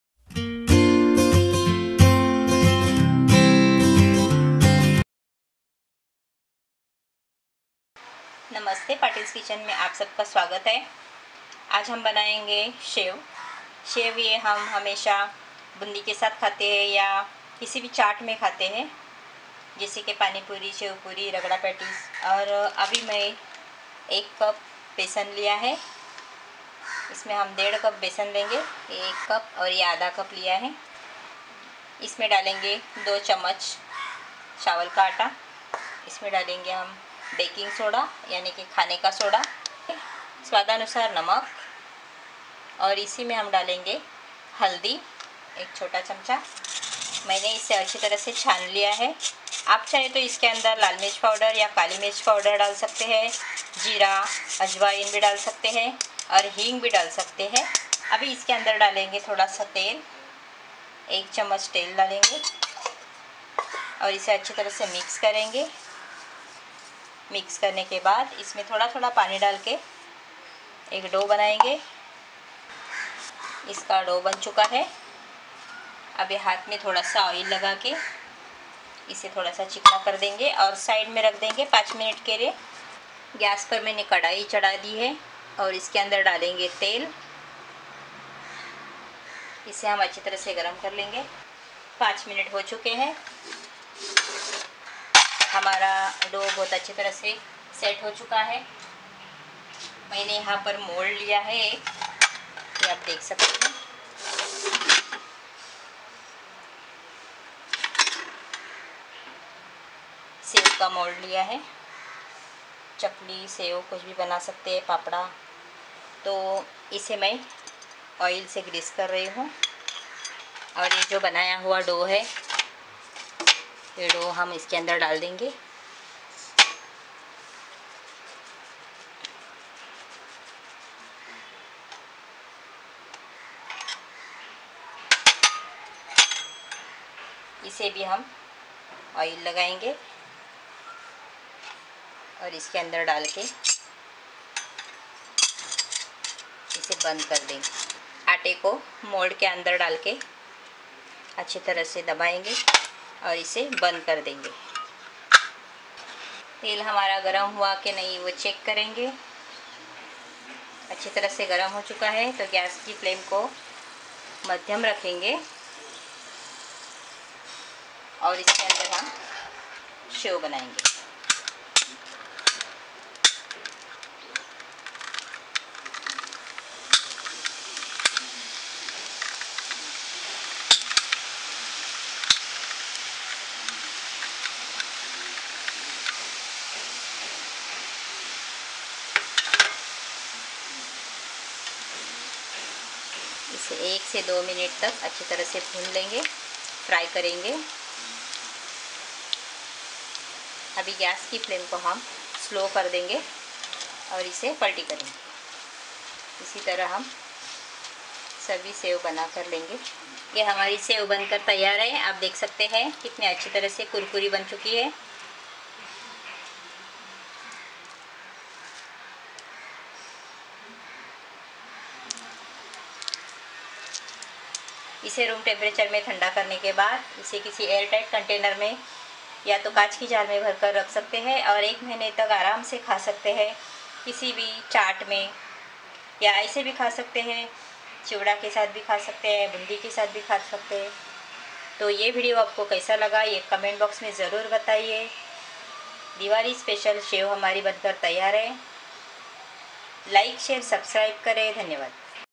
Crispy Shev | Diwali Recipe नमकिन कुरकूरी सेव घरपर आसानीसे कैसे बनाये वो देखिये.
INGREDIENTS:
 1&1/2 cup Besan
 2tbs Rice Flour
 1/2ts Baking Soda
 Salt to taste
 1ts Turmeric
 1tbs Oil

METHOD:
• In a bowl, take besan. Add rice flour, salt, turmeric powder and baking soda. Mix everything well.
• Add water and make thick dough out of it. Much water is not required. 4-5 tbsp water is sufficient.
• Knead the dough. Brush a little oil so that the dough won’t be dry. Cover and give it a standing time of 5 minutes.
• Take chakali mould and fit the shev disc in it.
• Roll the shev dough in cylindrical shape and fill the mould with the dough.
• Press the dough in the mould. Close the mould.
• Heat oil in a pan. When oil is enough hot, drop the shev into hot oil.
• Oil should not be too hot while dropping the shev. Fry the shev on medium heat for 3-4 minutes.
• After 2 minutes, flip the shev over with the help of fork.
• Fry from another side for about 2 minutes.
• Don’t drop the shev in too hot oil and fry it on medium heat.
• Or shev will be over fried and it’s colour may change.
• Shev should be nice yellow in colour.
• Remove the shev into a tissue paper.
• When shev cools down completely store it in airtight container.
• It has shelf life of about a month.